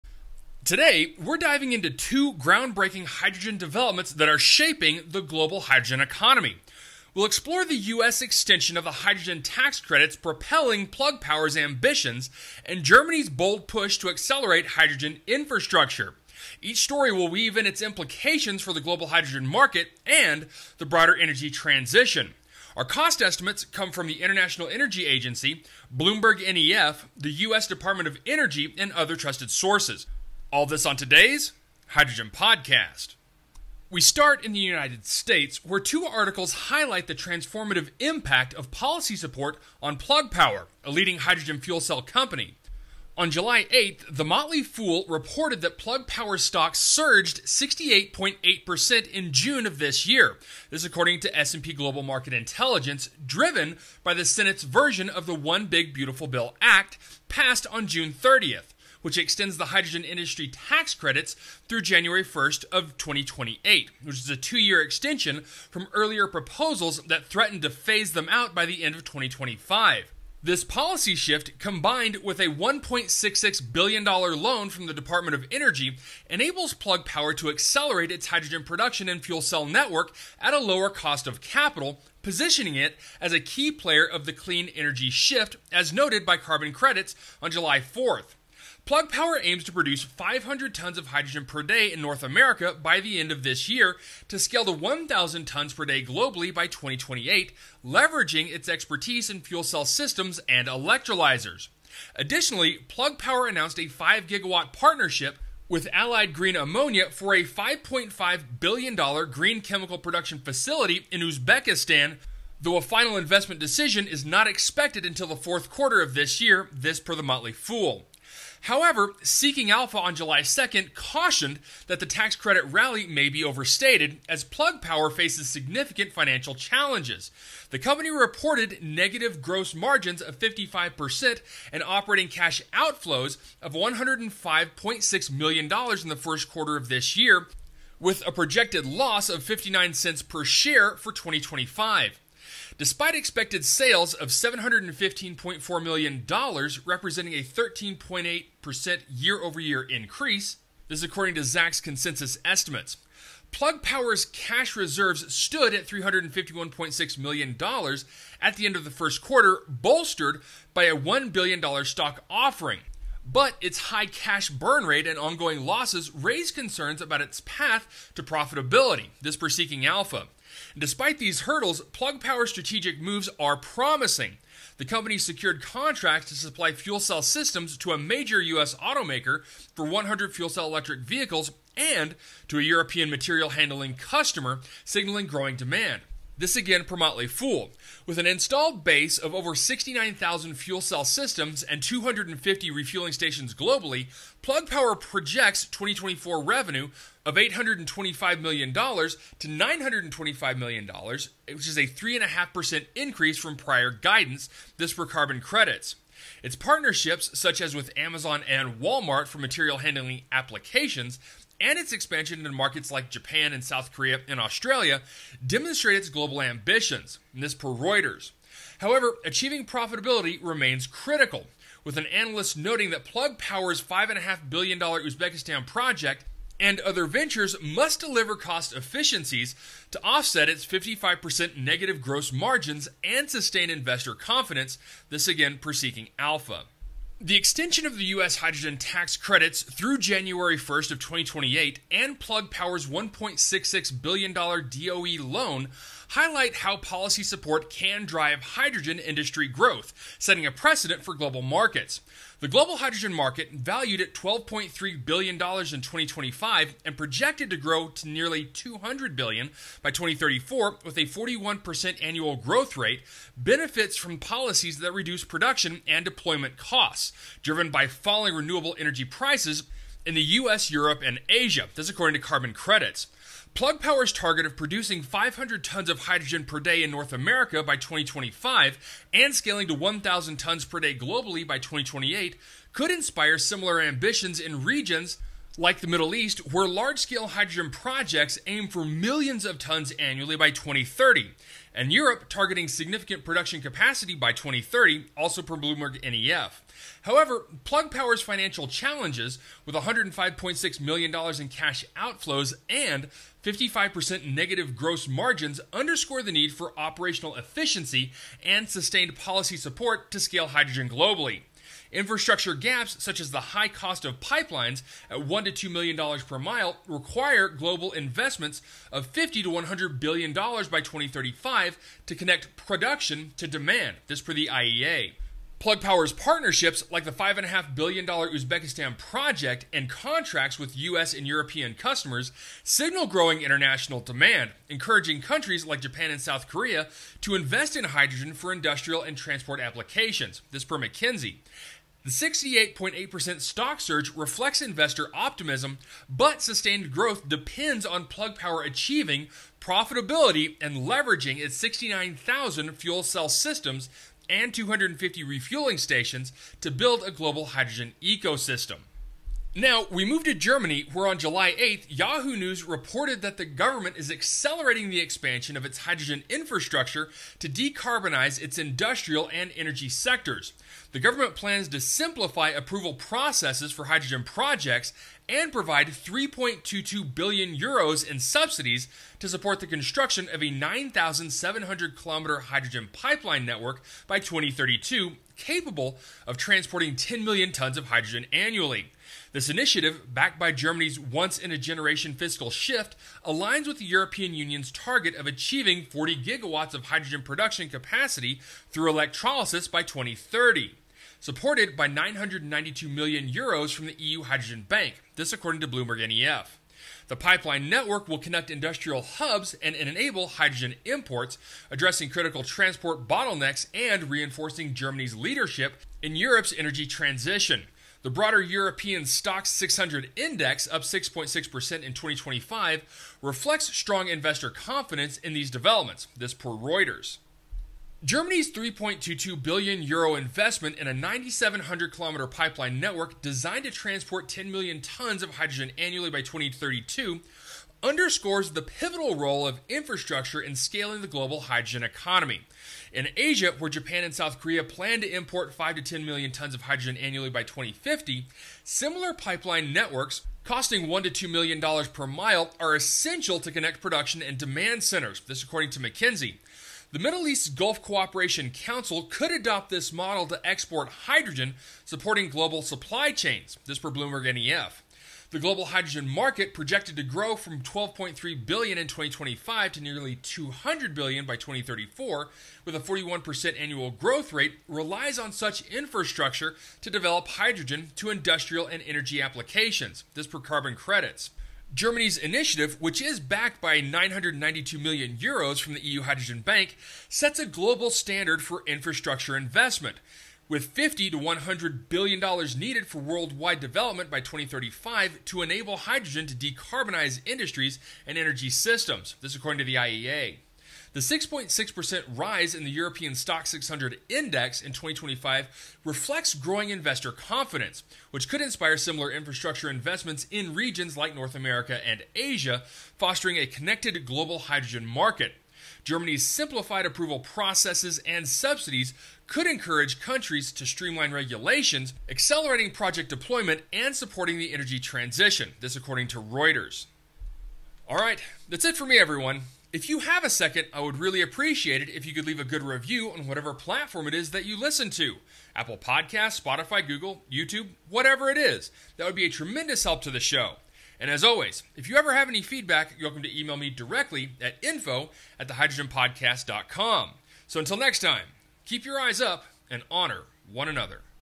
Plug Power Surges & Germany Goes Big: A Hydrogen Wake-Up Call
Welcome back to The Hydrogen Podcast! I’m Paul Rodden, and today we’re breaking down two major hydrogen headlines that could reshape the global energy landscape:

🇺🇸 Plug Power’s Stock Skyrockets After U.S. Tax Credit Extension
🇩🇪 Germany Unveils €3.2 Billion Plan for a 9,700-Kilometer Hydrogen Pipeline

In this episode, you’ll discover:

🔹 How the U.S. Senate’s extension of hydrogen tax credits through 2028 unlocked Plug Power’s $1.66 billion DOE loan.
🔹 The scale and ambition behind Plug’s 500 TPD U.S. goal and its $5.5B Uzbekistan partnership.
🔹 Germany’s plan to transport 10 million tons of hydrogen annually via its new pipeline network.
🔹 The financial challenges Plug still faces—like negative margins and high cash burn.
🔹 What this all means for the $200 billion global hydrogen market by 2034.

📊 Supported by data from the IEA, DOE, BloombergNEF, McKinsey, and more, we explore how infrastructure and policy are colliding to ignite the hydrogen transition.